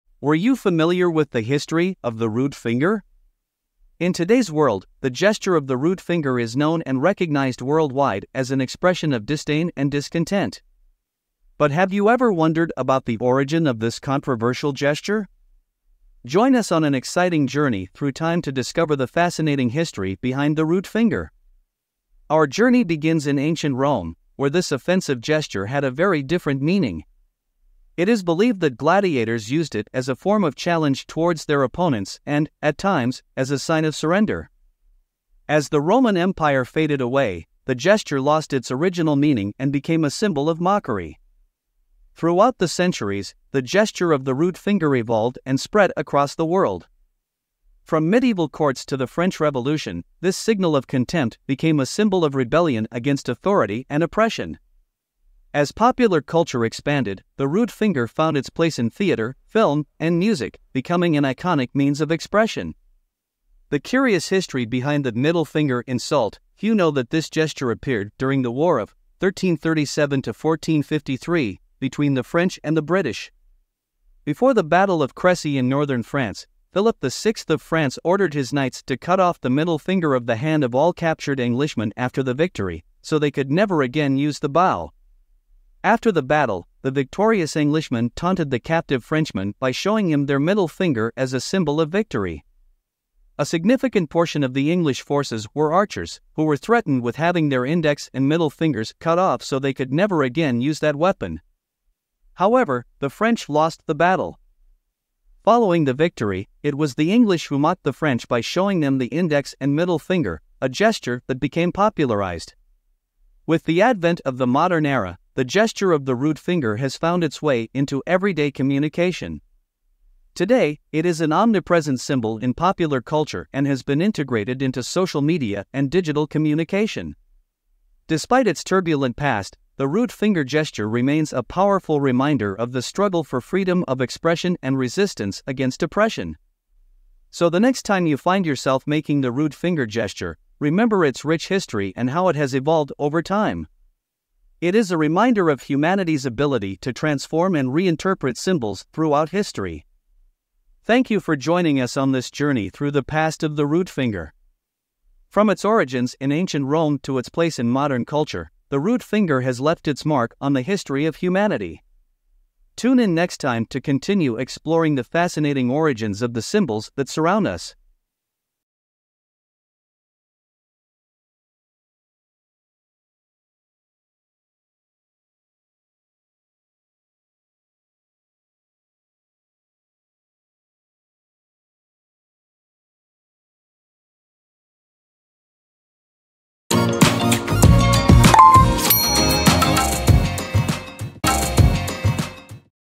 Were you familiar with the History of the Rude Finger?
In today's world, the gesture of the rude finger is known and recognized worldwide as an expression of disdain and discontent. But have you ever wondered about the origin of this controversial gesture? Join us on an exciting journey through time to discover the fascinating history behind the rude finger.

Our journey begins in ancient Rome, where this offensive gesture had a very different meaning. It is believed that gladiators used it as a form of challenge towards their opponents and, at times, as a sign of surrender. As the Roman Empire faded away, the gesture lost its original meaning and became a symbol of mockery.

Throughout the centuries, the gesture of the rude finger evolved and spread across the world. From medieval courts to the French Revolution, this signal of contempt became a symbol of rebellion against authority and oppression. As popular culture expanded, the rude finger found its place in theater, film, and music, becoming an iconic means of expression.

The curious history behind the "middle finger" insult: Few know that this gesture appeared during the war of (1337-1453) between the French and the British. Before the Battle of Crécy in northern France, Philip VI of France ordered his knights to cut off the middle finger of the hand of all captured Englishmen after the victory, so they could never again use the bow. After the battle, the victorious Englishmen taunted the captive Frenchman by showing him their middle finger as a symbol of victory. A significant portion of the English forces were archers, who were threatened with having their index and middle fingers cut off so they could never again use that weapon. However, the French lost the battle. Following the victory, it was the English who mocked the French by showing them the index and middle finger, a gesture that became popularized.

With the advent of the modern era, the gesture of the rude finger has found its way into everyday communication. Today, it is an omnipresent symbol in popular culture and has been integrated into social media and digital communication. Despite its turbulent past, the rude finger gesture remains a powerful reminder of the struggle for freedom of expression and resistance against oppression.

So the next time you find yourself making the rude finger gesture, remember its rich history and how it has evolved over time. It is a reminder of humanity's ability to transform and reinterpret symbols throughout history. Thank you for joining us on this journey through the past of the rude finger.

From its origins in ancient Rome to its place in modern culture, the rude finger has left its mark on the history of humanity. Tune in next time to continue exploring the fascinating origins of the symbols that surround us.